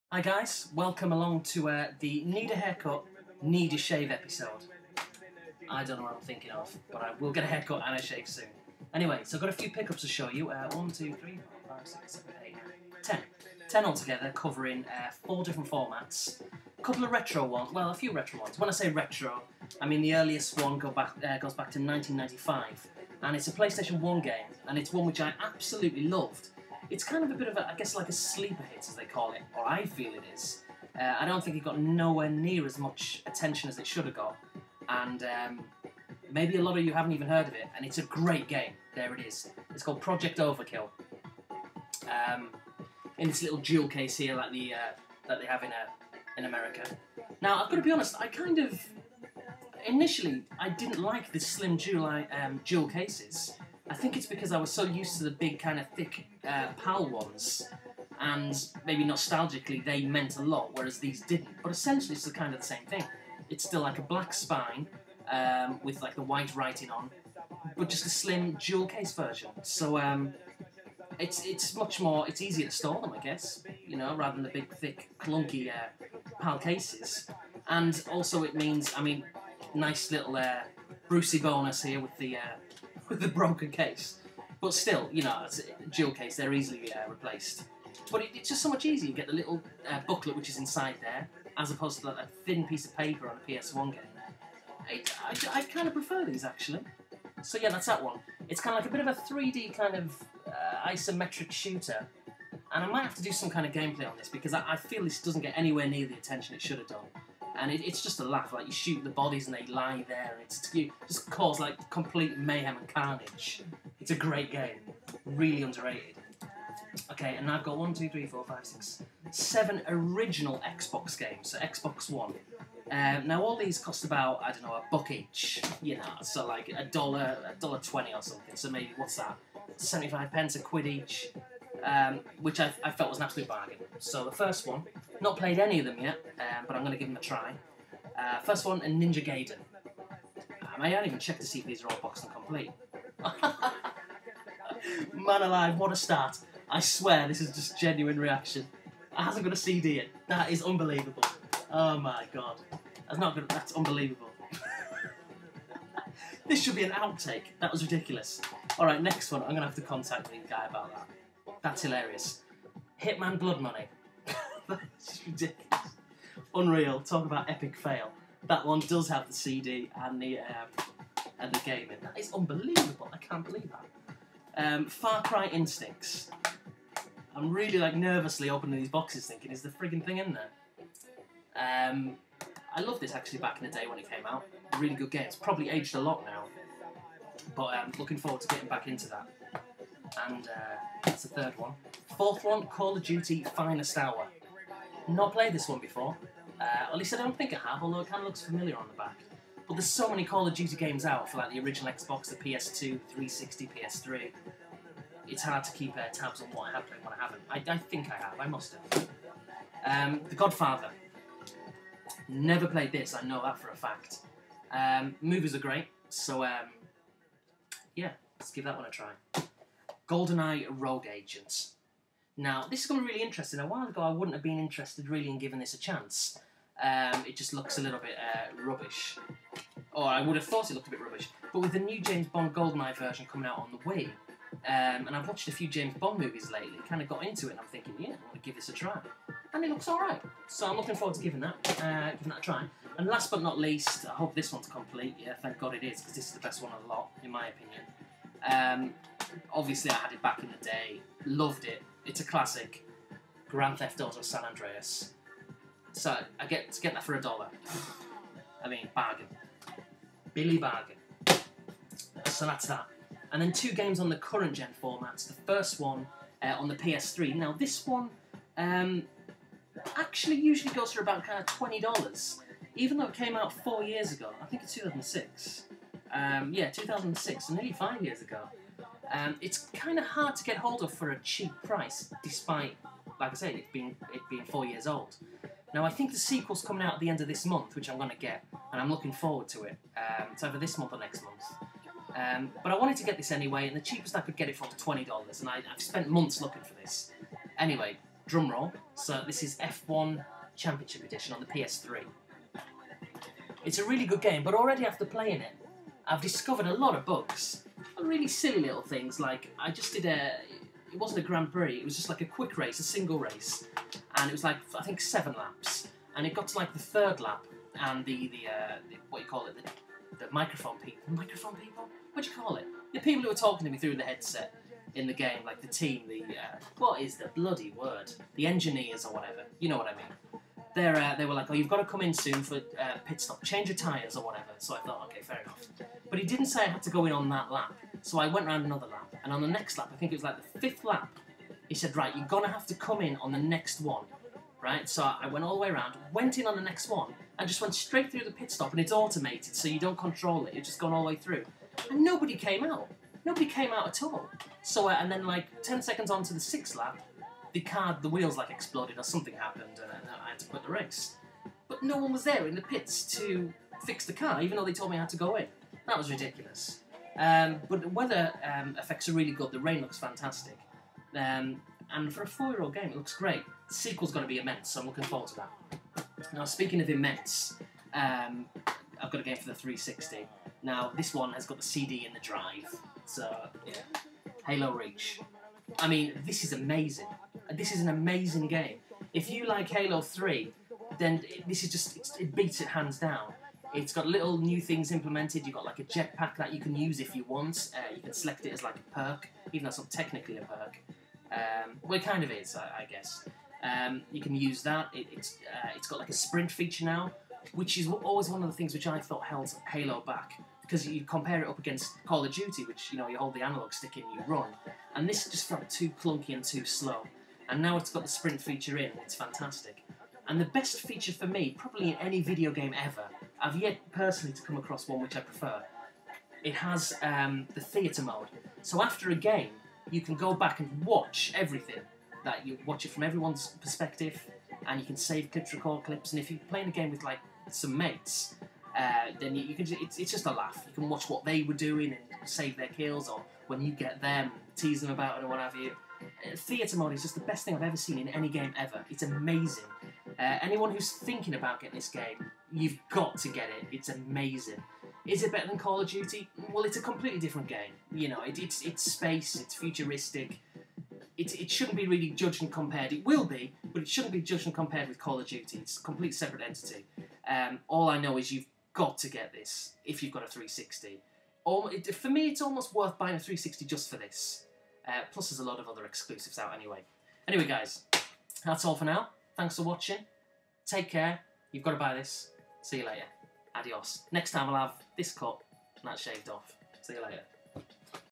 Latest pickups - mid September
Few more additions to the collection. PS1, XBOX, 360 and PS3.